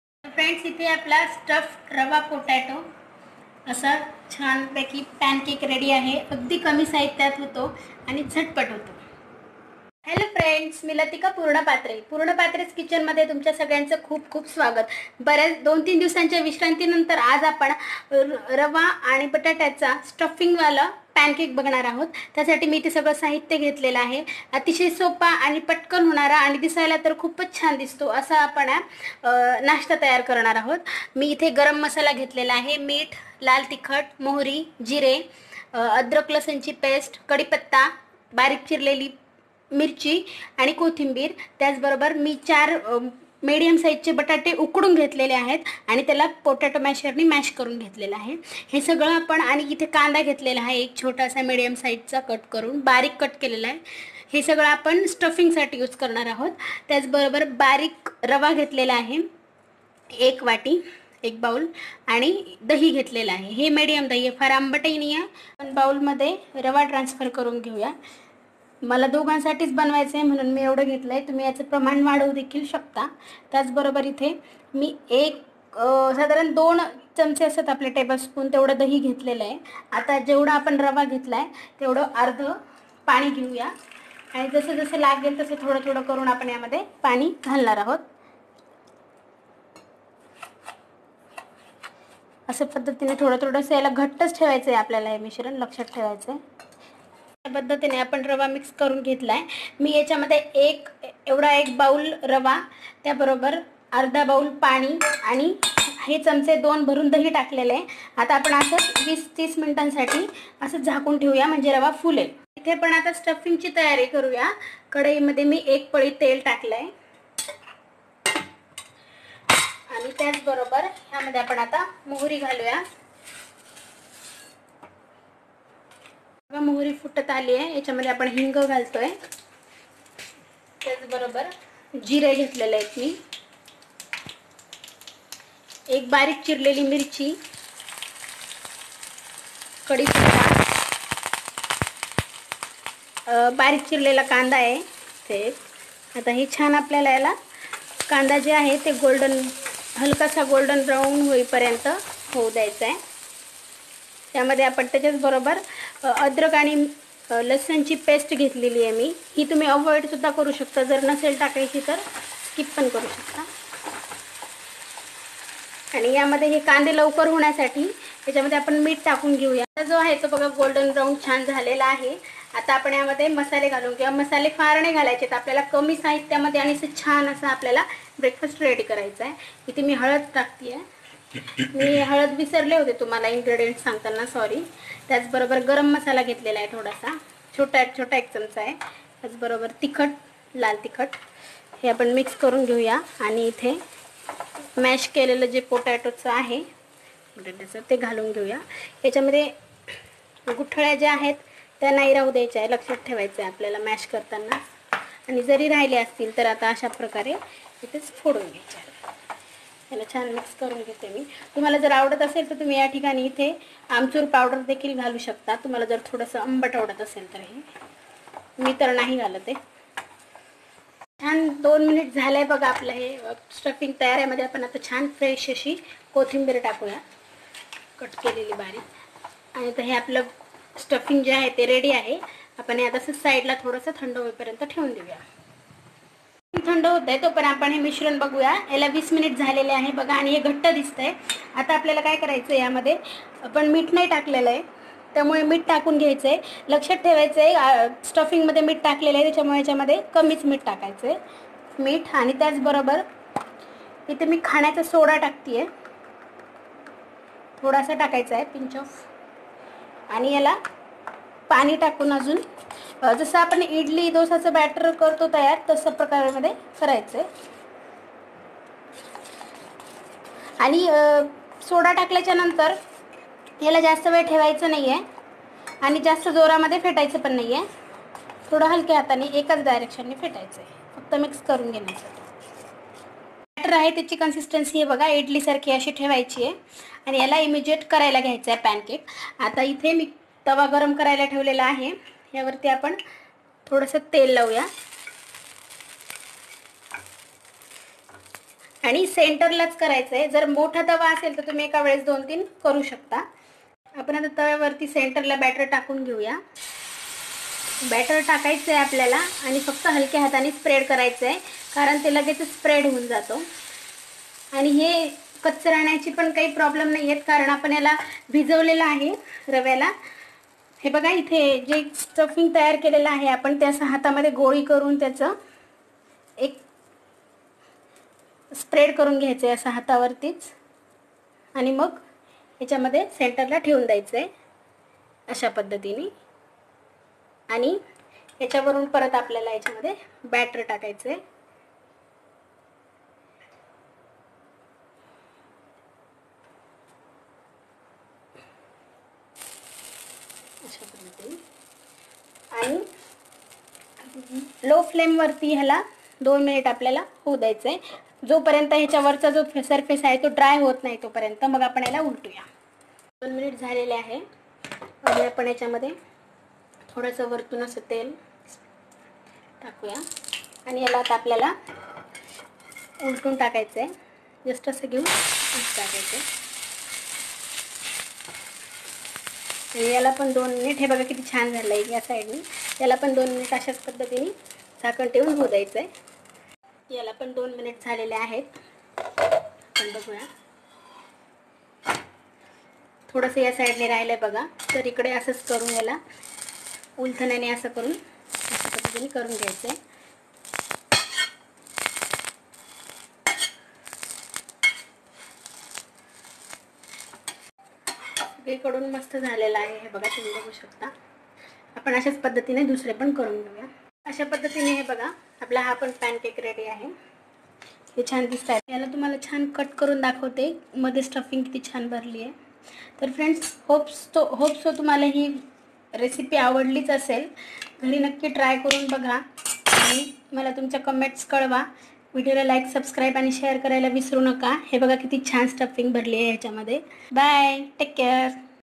||Best breakfast stuffed potato suji pancake|| फक्त रवा आणि बटाटा पासून पटकन आणि कमी तेलात नाश्ता
Rava Potato stuffed pancake

It's an oil free and quick recipe.using sujji/samolina/ rava and mashed potato masala.

Ingredients

Suji/rava -1 cup
Boiled potatoes-4
green chillies-2
Curry leaves- 10
Onion-1
Cooking oil-3tsp

breakfast recipe,snacks recipe,tea time snacks,sooji recipe,aloo recipe,aloo aur suji ka nashta, suji ka nashta, aloo ka nashta